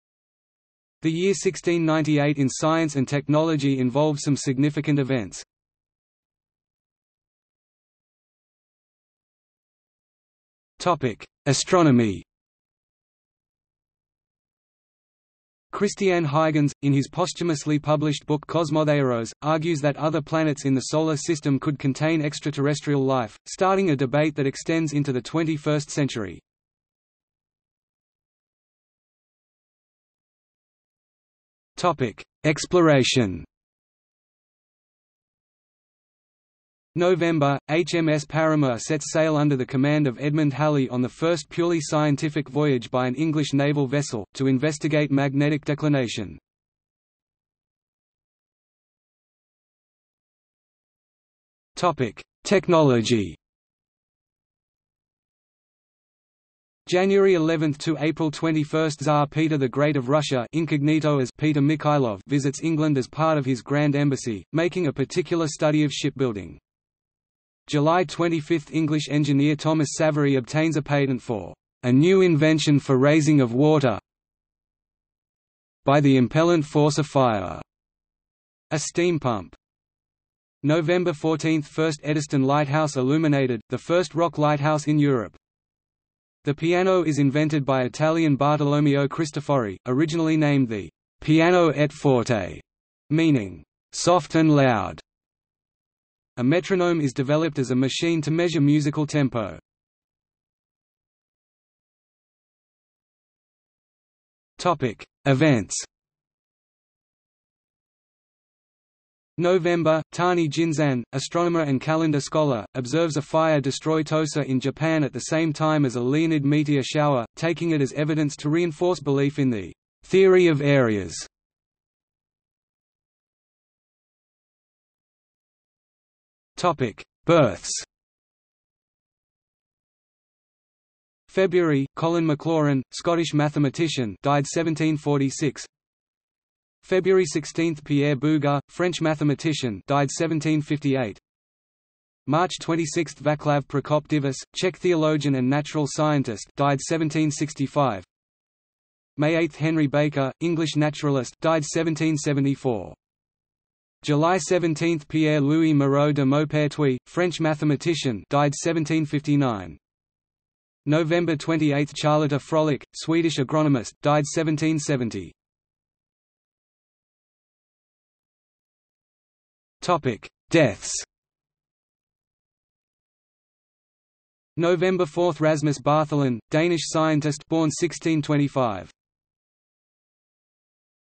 1698 in science | Wikipedia audio article
00:00:07 1 Astronomy
00:00:28 2 Exploration
00:00:48 3 Technology
00:01:41 4 Events
00:02:03 5 Births
00:02:46 6 Deaths

- Socrates